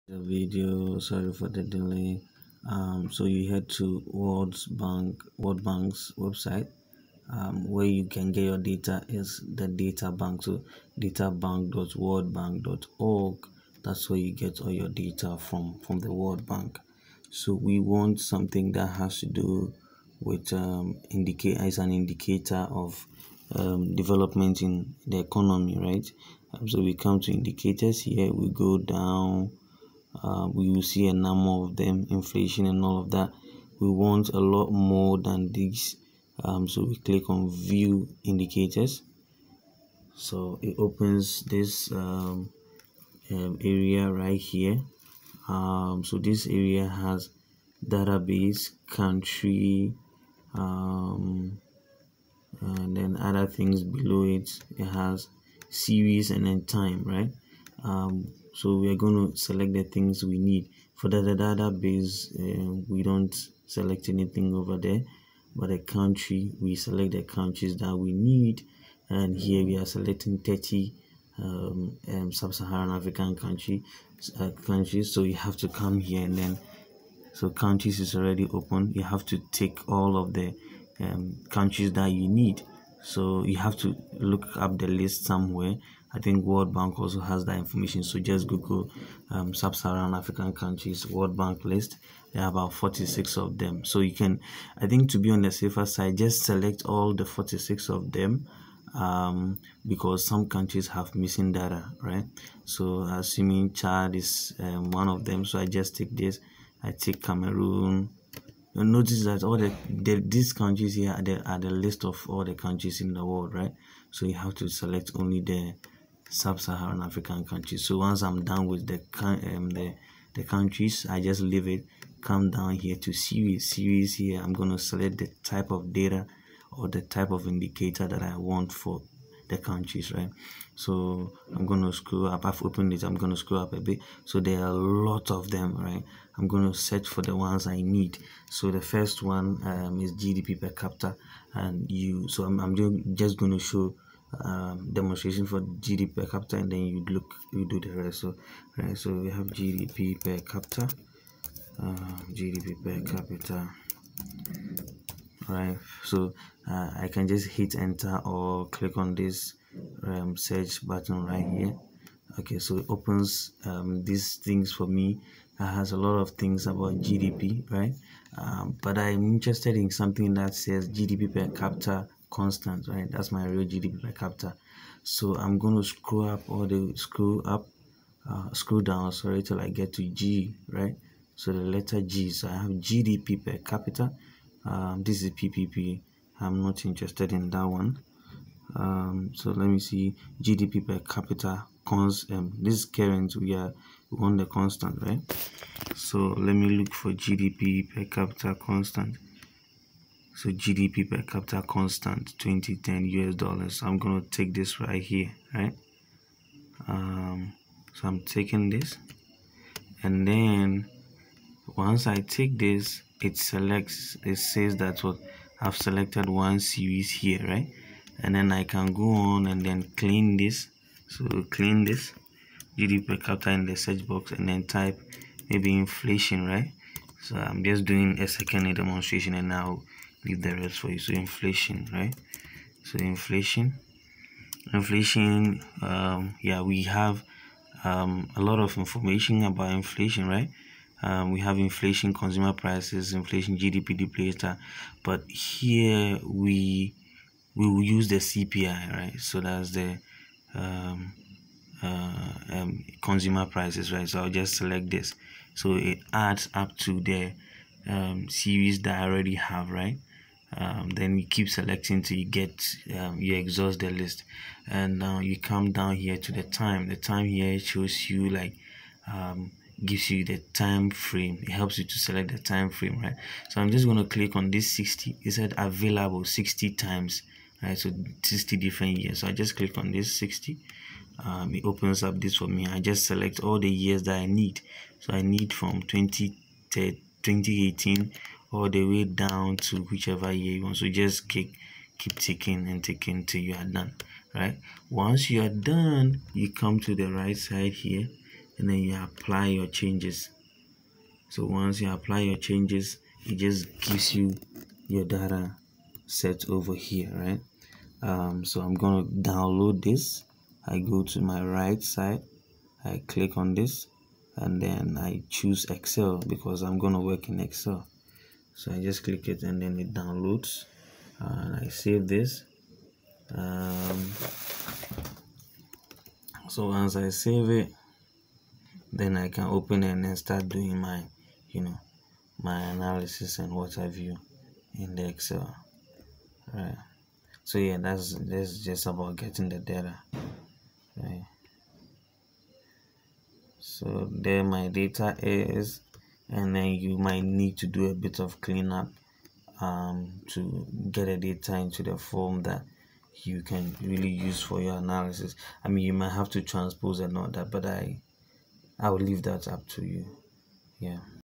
Downloading Economic Data from WorldBank
This video demonstrates how to get Economic Data from WorldBank's website.